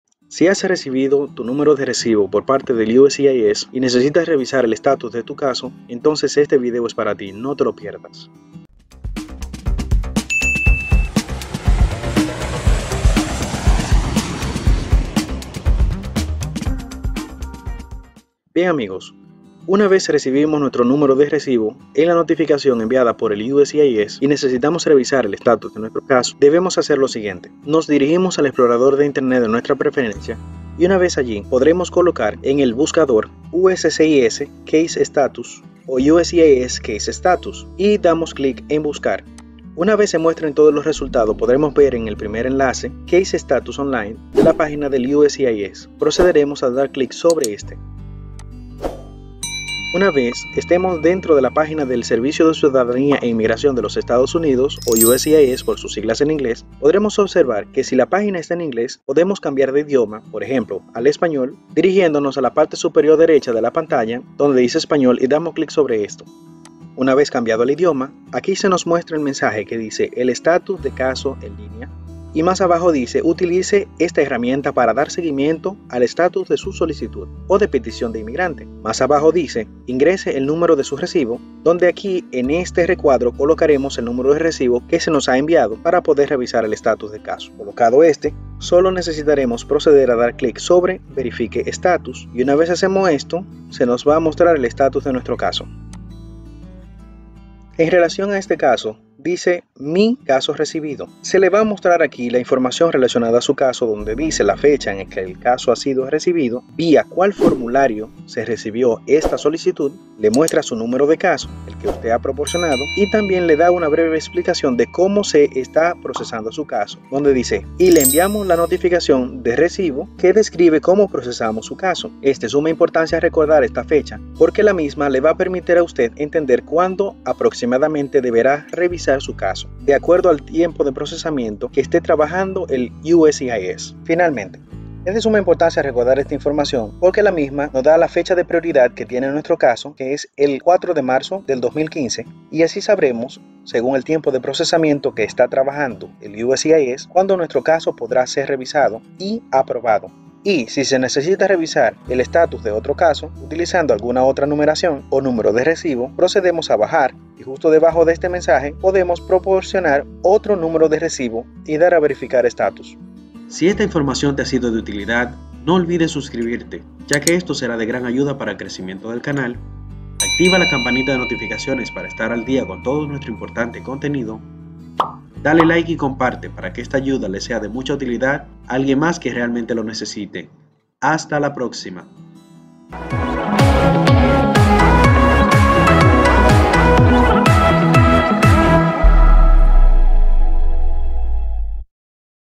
Aprende a revisar el estatus de tu caso en el USCIS. No te lo pierdas.
En este video se les muestra en simples pasos como revisar el estatus de un caso de peticion presentado al USCIS al recibir su numero de recibo de su solicitud.

Espero esta información te sea de mucha utilidad para avanzar en tu caso. Hasta la próxima.